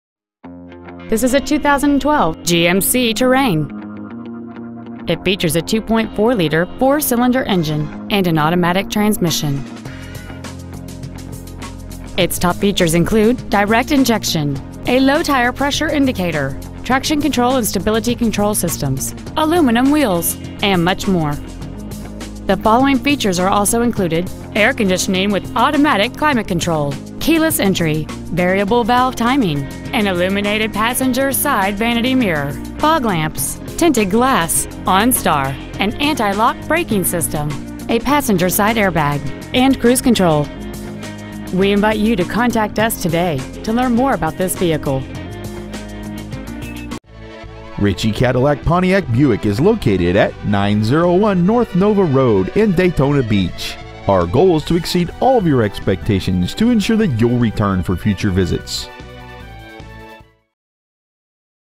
Preowned 2012 GMC Terrain Daytona Beach FL 32117
Year : 2012
 Make : GMC
 Model : Terrain
 Engine : 2.4L DOHC 4-cylinder SIDI (Spark Ignition Direct Injection)
 Trans . : Automatic
 Exterior : Black
 Miles : 5